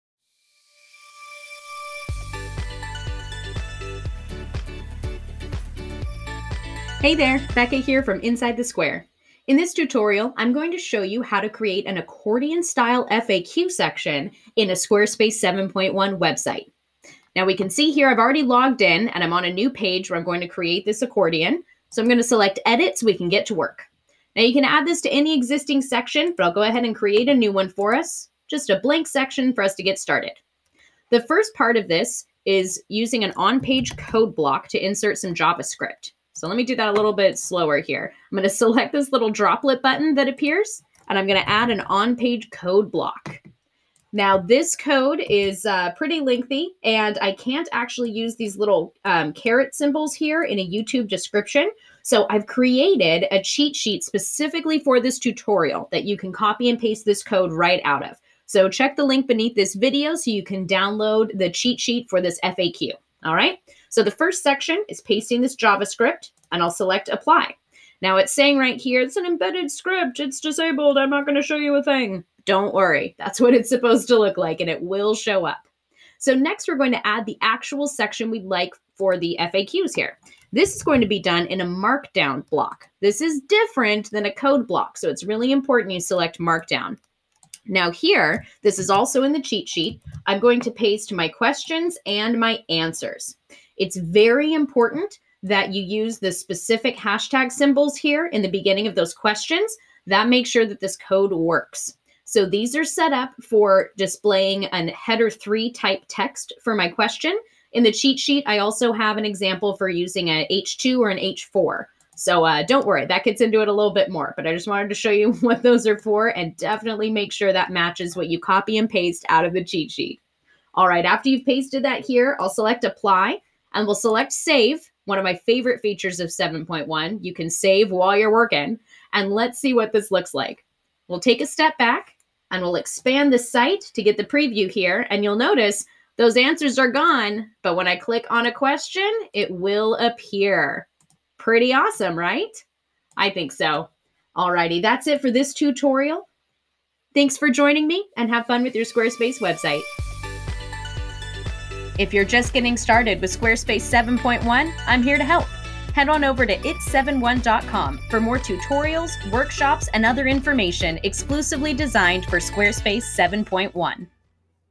How to create an accordion FAQ in Squarespace 7.1 // Squarespace 7.1 Tutorial
This tutorial will show you how to create expandable sections, also called "accordion" sections in Squarespace using some fancy code.

The idea behind this layout effect is that you only have some text displayed, and then when someone clicks on the headline, additional text will be displayed underneath it. It is really common to see these used for FAQ sections on a site; you click on the question and the longer answer will show up underneath it.

To create this clickable action, we are using a combination of two on-page elements - a markdown block that will contain all the info we want to display on the site, and an on-page code block that will have the "Script" telling a browser what to do with our content.

This one is a complicated two-parter so I created a cheat sheet with all the code options for you to copy and paste from. To grab the info head on over